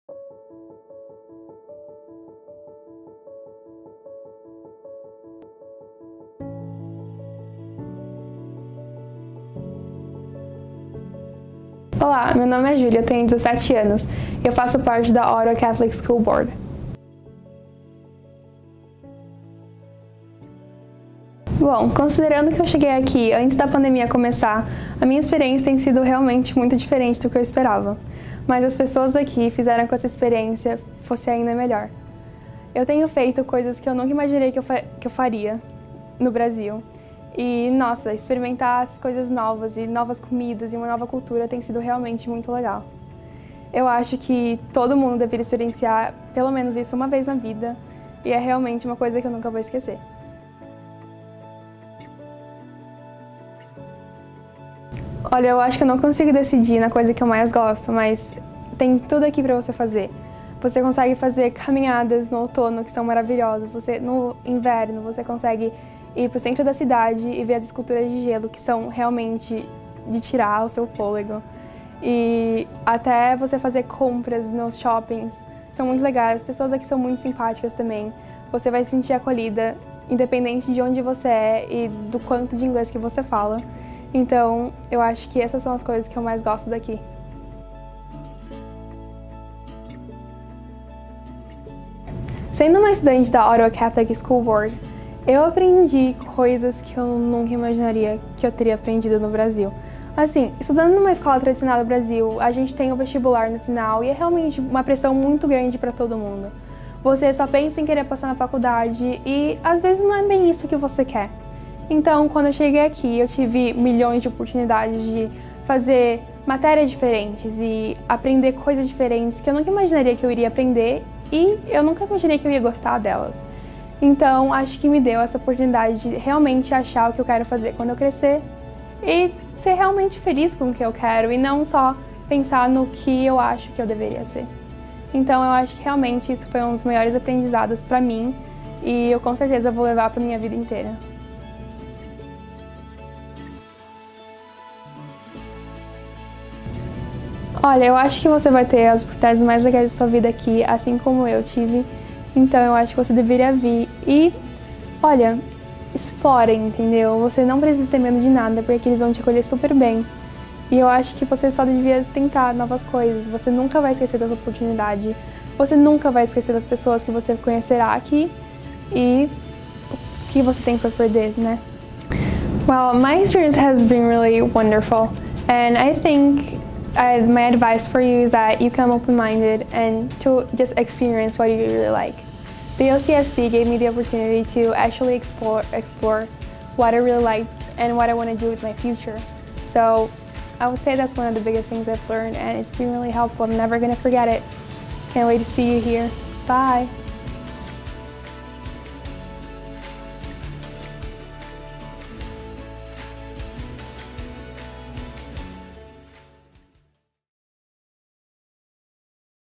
Julia's experience at the Ottawa Catholic School Board (Portuguese)
International student Julia, shares her experience at the Ottawa Catholic School Board (Portuguese).